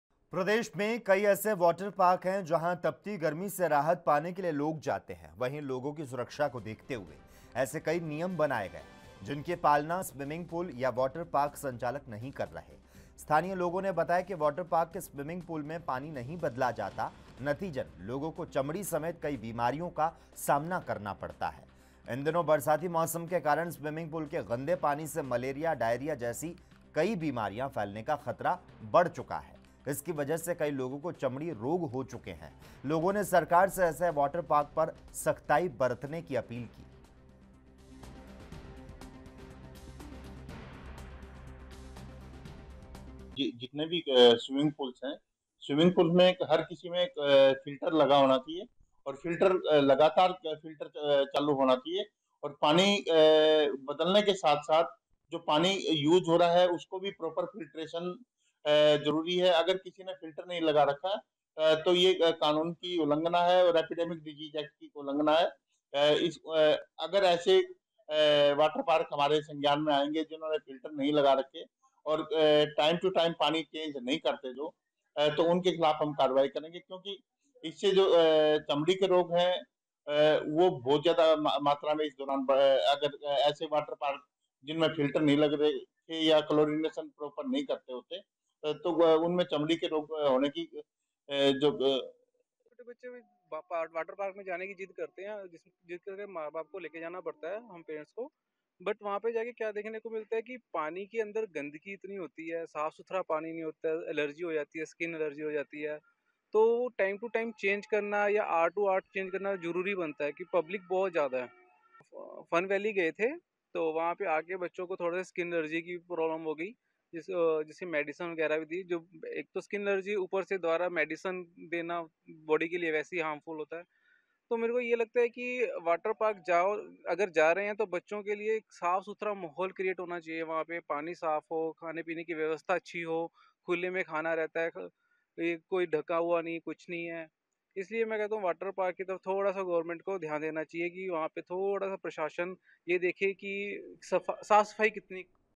वाटर पार्क संचालक नहीं कर रहे नियमों का पालन ! बरसाती मौसम में बढ़ रहा बीमारियों का खतरा | ANB NEWS
प्रदेश में कई ऐसे वॉटर पार्क हैं जहां तपती गर्मी से राहत पाने के लिए लोग जाते हैं। वहीं लोगों की सुरक्षा को देखते हुए ऐसे कई नियम बनाए गए हैं, जिनकी पालना स्विमिंग पूल या वॉटर पार्क संचालक नहीं कर रहे हैं...स्थानीय लोगों ने बताया कि वाटर पार्कों के स्विमिंग पुलों में पानी नहीं बदला जाता है, नतीजन लोगों को चमड़ी समेत कई बीमारियों का सामना करना पड़ता है...इन दिनों बरसाती मौसम के कारण...स्विमिंग पुलों के गंदे पानी से मलेरिया, डायरिया जैसी कई बीमारियां फैलने का खतरा बढ़ गया है...इसकी वजह से कई लोगों को चमड़ी रोग हो चुके है...लोगों ने सरकार से ऐसे वाटर पार्कों पर सख्ताई बरतने की अपील की है...